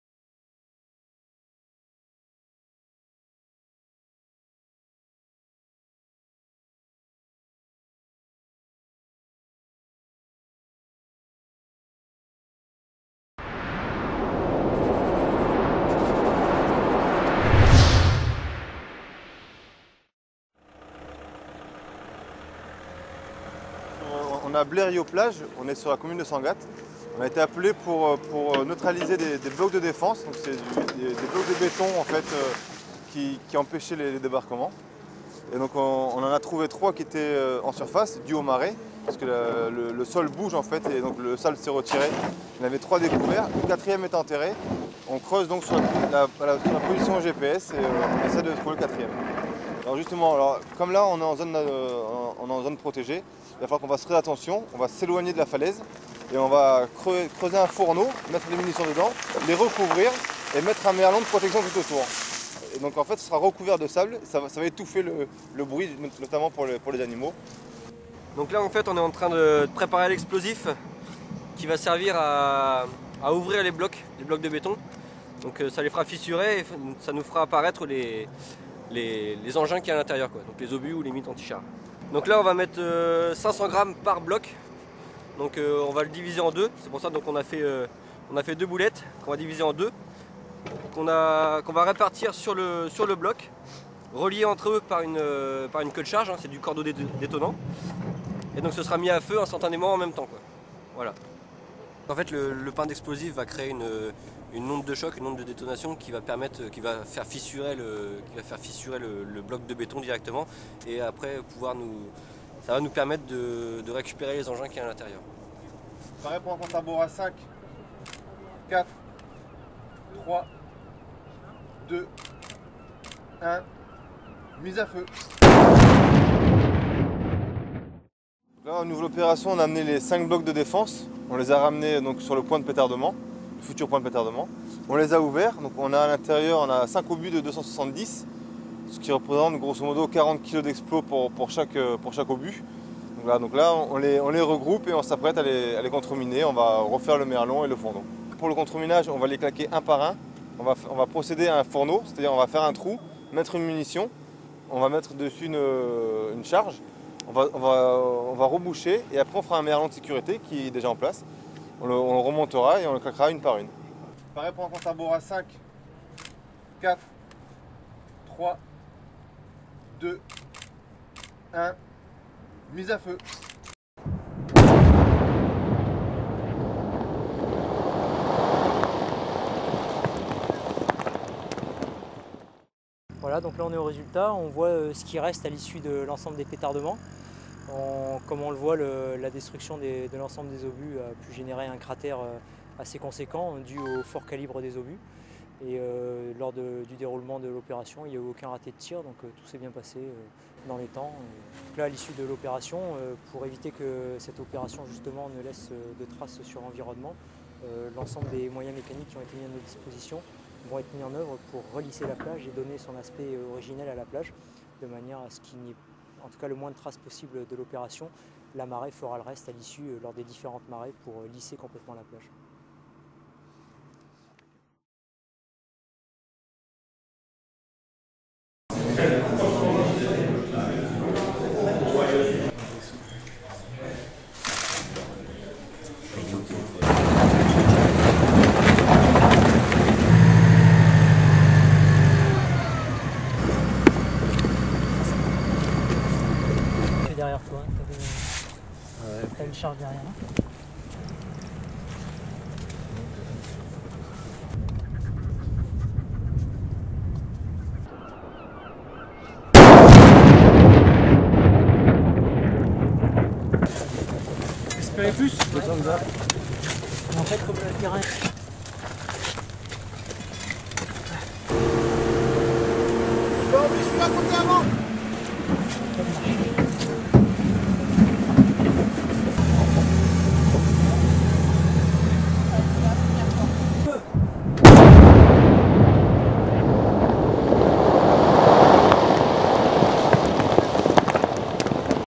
deminage nussknacker sangatte
vidéo et photo de la marine nationale , sur un déminage de Nussknacker sur la plage de Sangatte en décembre 2013